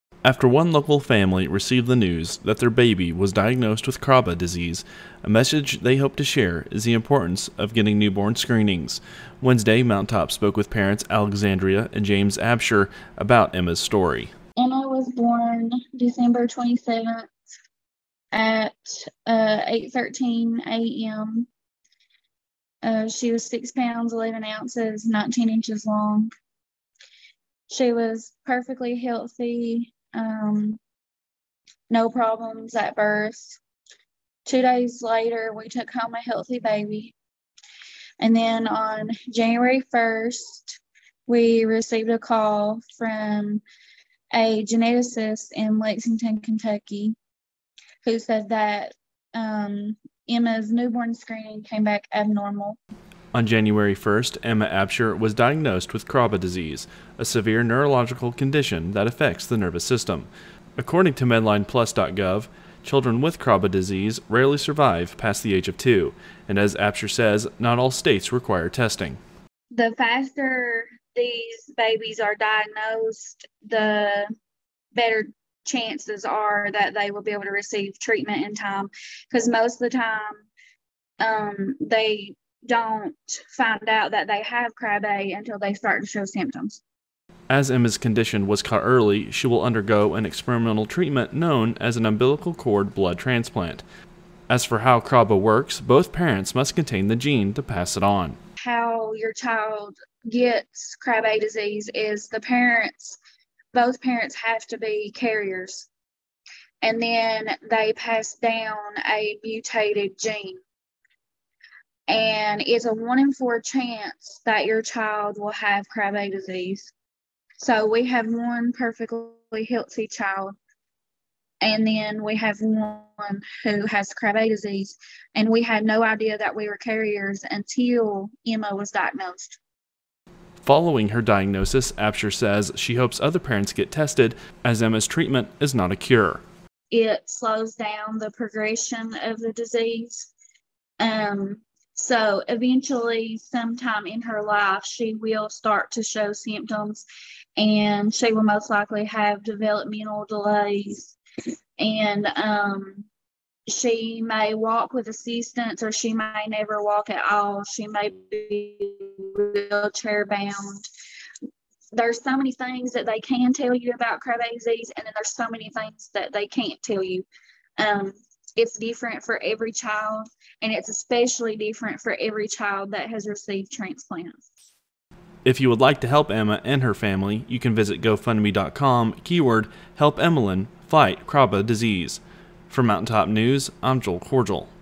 Family Advocates for Newborn Screenings
After one local family received the news that their baby was diagnosed with Krabbe Disease, they hope to share a message on the importance of getting screenings for newborns.